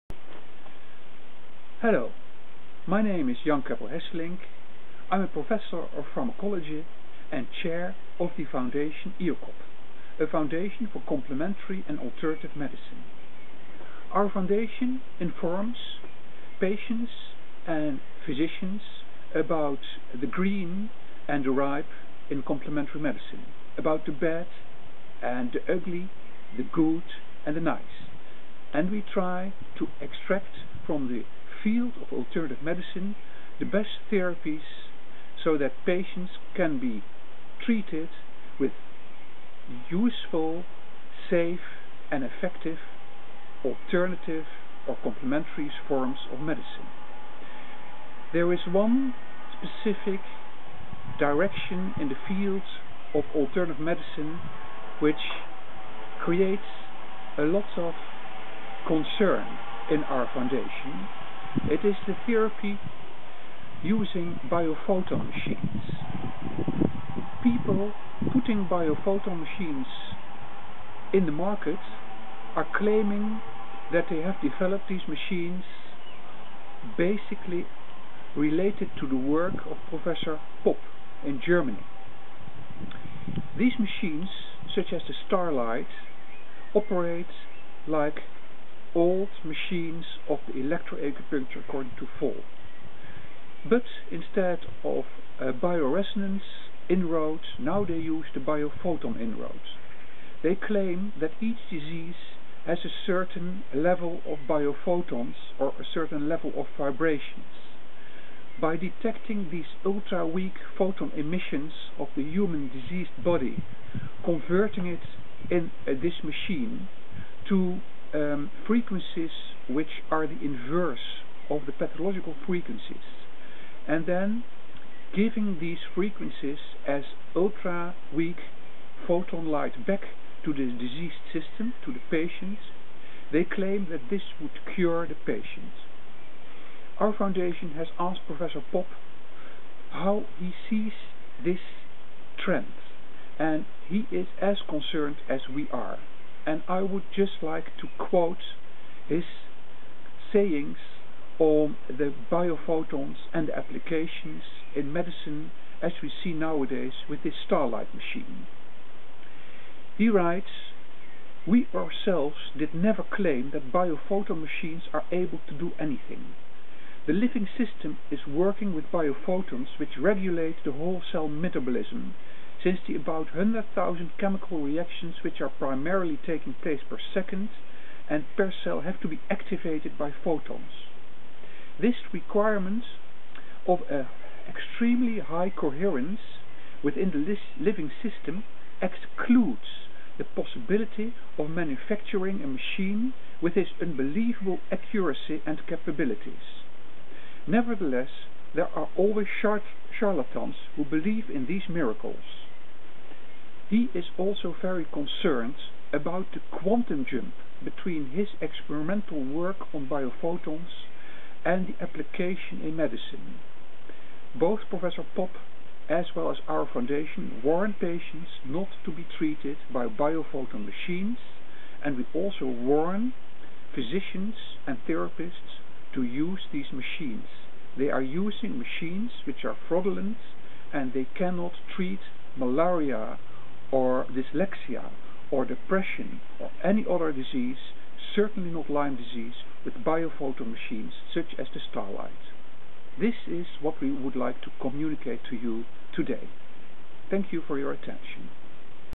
Biophotons: a critical appraisal by Foundation IOCOB
Biophotons: a critical appraisal by Foundation IOCOB. Prof.dr. Jan M. Keppel Hesselink, MD and chair of the foundation IOCOB discusses the problematic biophoton therapy, and he also gioves quotes from Dr Popp, the father of the biophoton work within plant physiology and other living systems. His concern is shared with us,  treatment of the Starlight of multiple sclerosis, malaria, depression, schizophrenia, to mention a few. This is unbelieveble, without any research done!

We herewith quote Dr Popp's concern:

We ourselves did never claim that "biophoton machines" are able at all. The living system is working with
biophotons which regulate the whole cell metabolism, since the about 100 000
chemical reactions which are  permanently taking place per second and per
cell have to be activated by photons (according to the knowledge of biochemistry, see, for instance, the text books of biochemistry). This requirement of a extremely high coherence within the living system EXCLUDES the possibility of manufacturing a machine with this unbelievable accuracy and capacities. Nevertheless, there are always scharlatenes who believe in these miracles.

IOCOB thinks it is very troublesome to read the indications, among whoch we even find malaria and multiple sclerosis!

And here the quotes of the startlight internetsite:

BIOSTARLIGHT

Welcome to Biostarlight, a Bio-Photon Coherency Therapy company that utilises StarLight technology to enable the body to heal itself. Biostarlight was founded in 2006 by Anouk Puper, a qualified Bio-Photon Therapist based in Chelsea, London.

WHAT IS BIO-PHOTON COHERENCY THERAPY?

Bio-Photon Coherency Therapy assists the body's immune system to function at optimum levels, which enables the body to heal itself. The Therapy utilises StarLight technology, which is a series of instruments capable of measuring light emissions from acupuncture points, using fibre-optic cables and crystal electrodes.

Once these light emissions or vibrations have been located the readings are fed into the system, which is fully equipped with therapy programmes based on common symptoms of other patient's experiences. The therapeutic process is then delivered using optical glass electrodes, held in the patient's hands.

WHAT CAN IT BE USED TO TREAT?

EXAMPLES

Scar Therapy:

Surgical scaring causes a great disturbance to the body, primarily in the blockage of the organ's energy supply. With the StarLight one can treat these scars and in about 50% of all cases, the scar disappears completely from the skin's surface. In all cases the scar tissue that has formed disappears along with its associated weather sensitivity and redness.

Spin Inversion Therapy:

An increasing number of people suffer from "electron spin inversion". This is caused by external magnetic fields that influence the rotational direction of the electrons in the body. As electromagnetic fields surround us in our everyday life (computers, television, electrical appliances...) the likelihood of electron spin inversion increases. The usual result is chronic fatigue, allergies and a body that works against itself. This therapy returns the electron spin to the proper direction.

Dyslexia Therapy:

There are many people who suffer from dyslexia, a non-integration of the brain hemispheres. This treatment treats more than just dyslexia.

Psychological Problems:

There are those who claim that physical problems stem from psychological disorders. It has been our experience that the reverse is true. Depression, manic depression, schizophrenia and phobias are all improved with the StarLight.